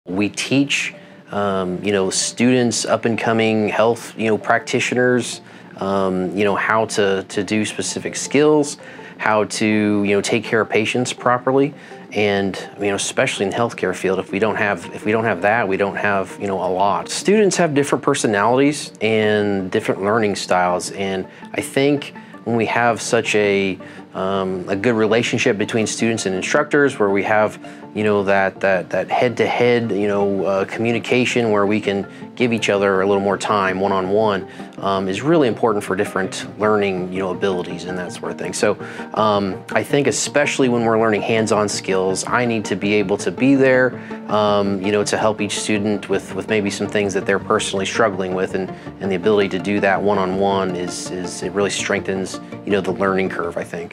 Dr. Jared Gullett on the Importance of Career and Technical Education
Lincoln Trail College Health Careers instructor Dr. Jared Gullett talks about the importance of a career and technical education as LTC celebrates Career and Technical Education Month.